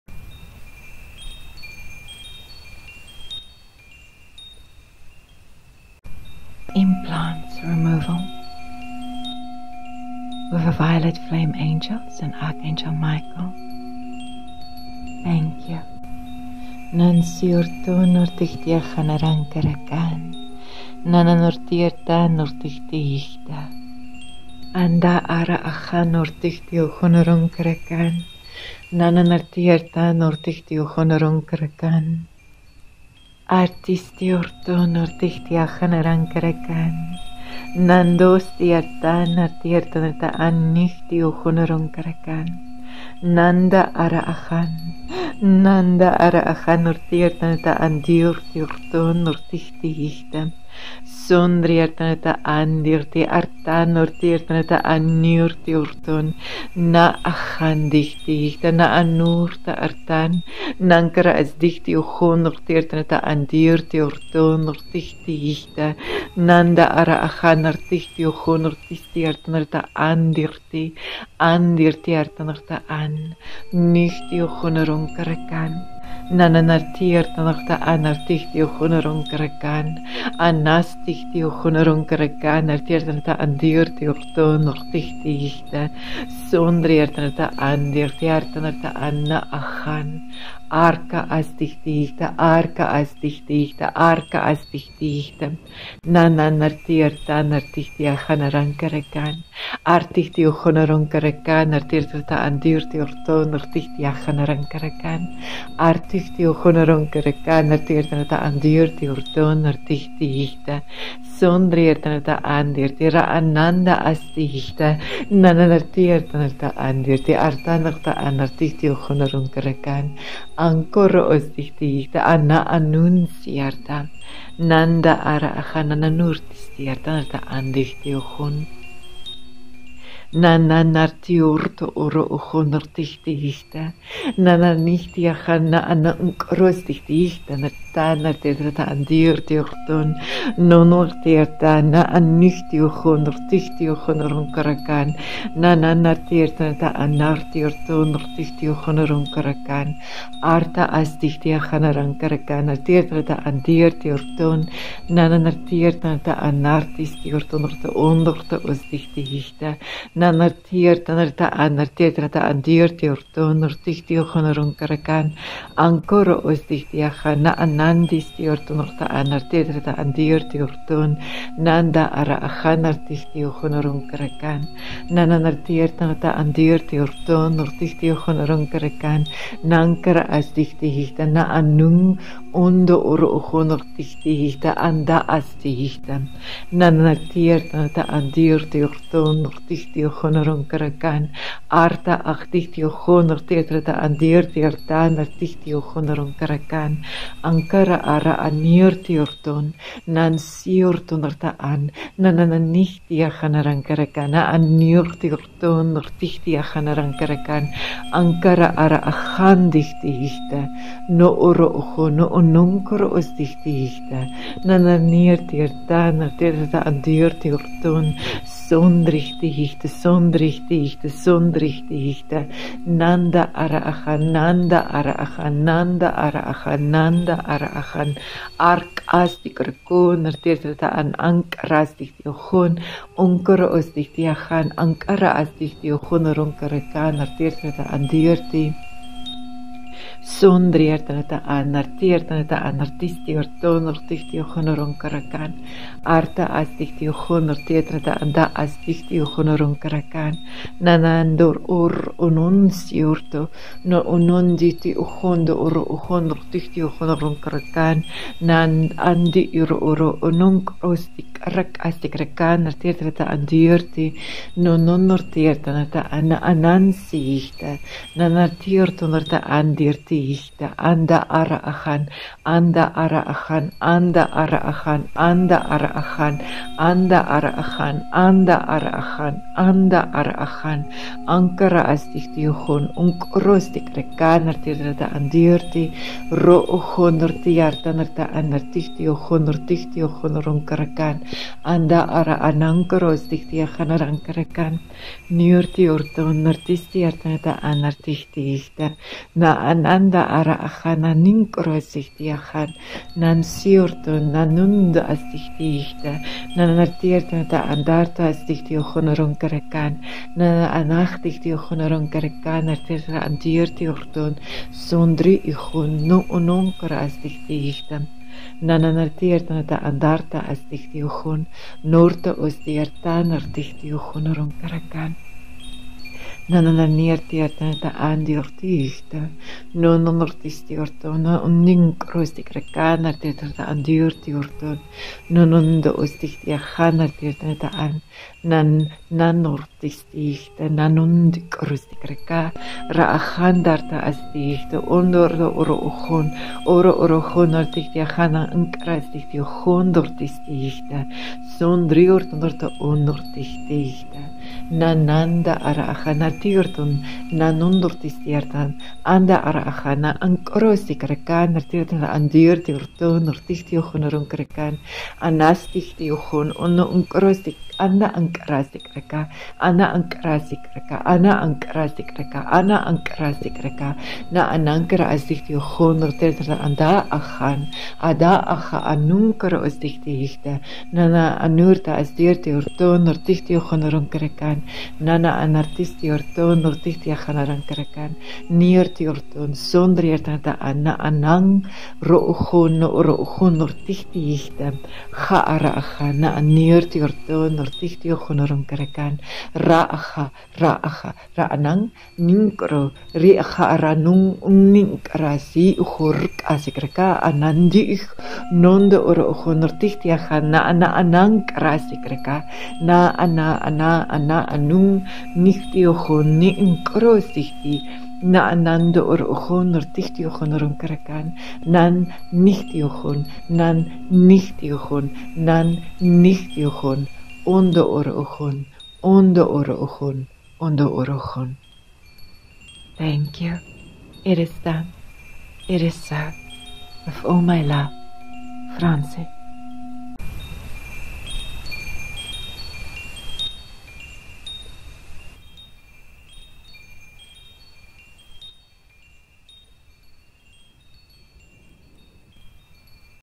Remove Implants Pure Light Language Reiki Channeling
Pure Light Language and Reiki to help us dismantle and revoke all agreements and vows that allow implants to remain in our energy field and bodies. A clearing of implants, especially those created through trauma and all the unintentional implants often formed through the relationships in our lives.
This is a healing that requires only an open heart and the willingness to receive. Be gentle with yourself afterwards as this may bring up things for you to transmute. With all my love, may you be happy and well.

Extended Healings:
Psychic Gifts Retrieval/ Become more Psychic
and more

Donations are always welcome and help me to continue to give to the collective for free, thank you!

For a private Reiki and/ Light Language Consultation or an Art Commission